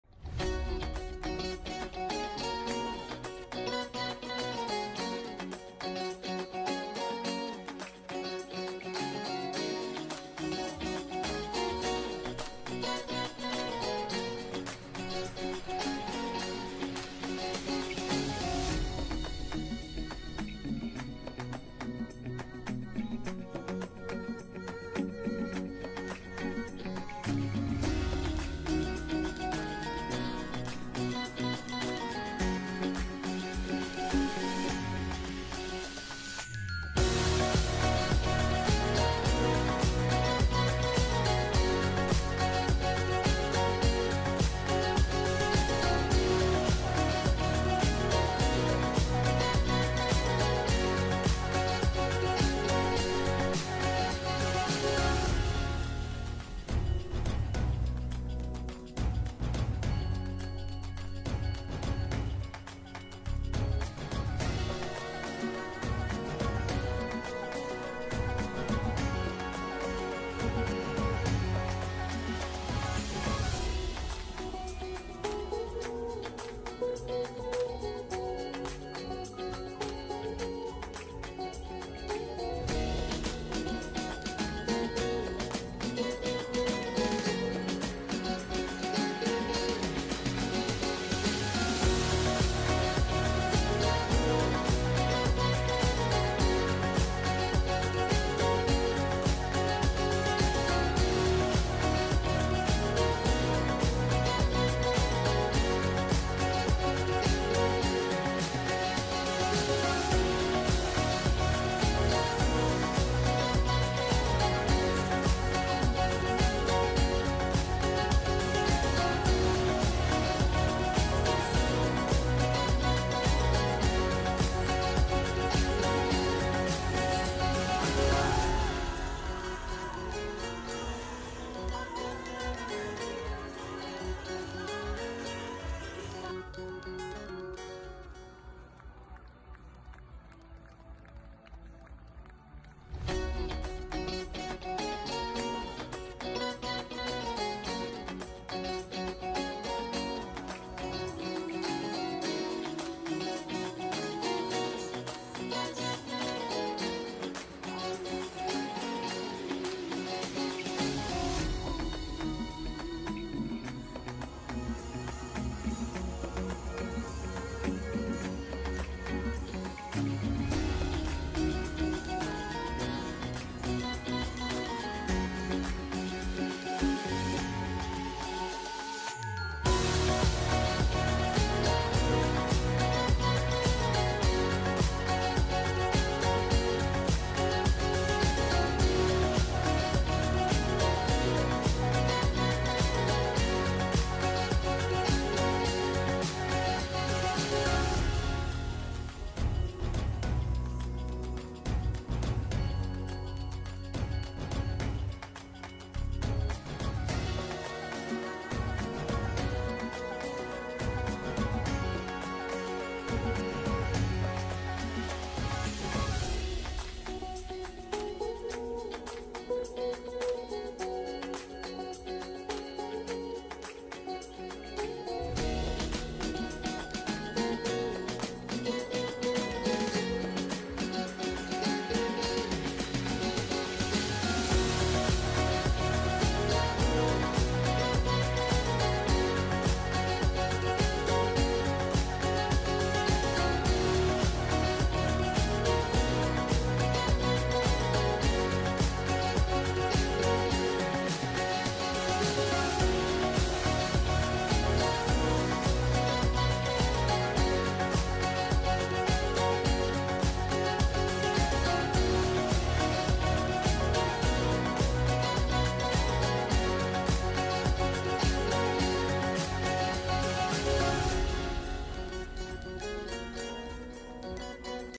WOW - The Vegas Spectacular Show Travel log
WOW - The Vegas Spectacular is pulling out all the stunts for one of the most thrilling 90-minutes you've ever experienced.